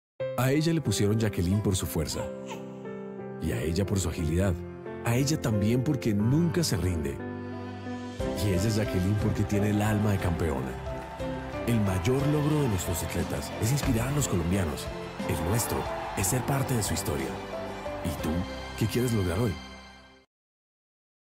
Jackeline Rentería - Claro Colombia
Nuestra doble medallista olímpica como parte de la campaña TambiénSoy de Claro Colombia. Jackeline Rentería